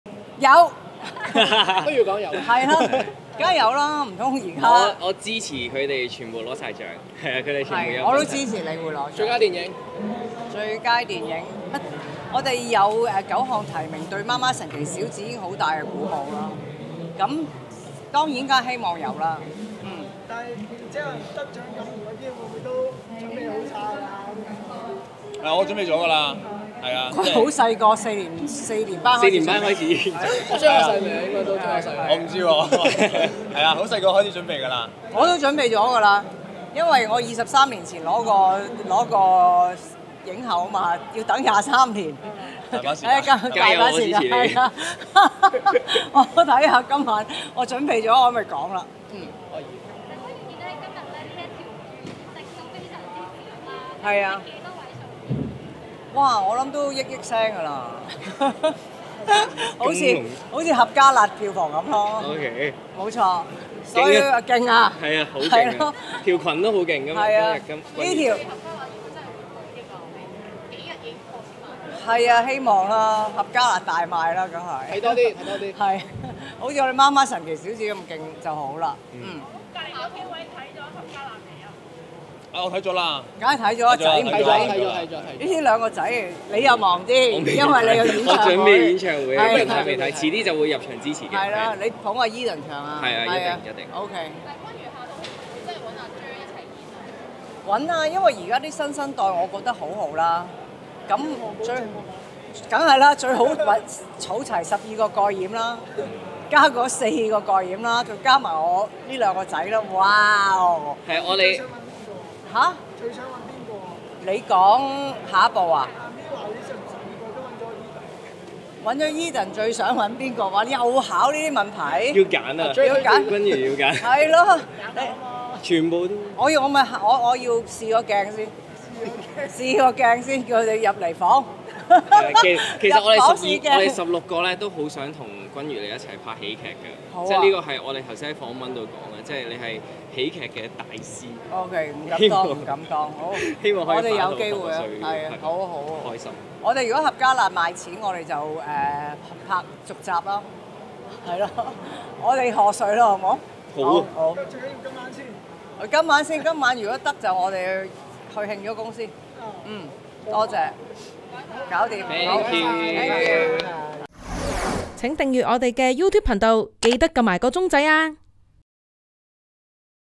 金像獎2022｜ 吳君如 Jer 馮皓揚 梁仲恆踏紅地氈 《媽媽的神奇小子》訪問
bmgproductionmusic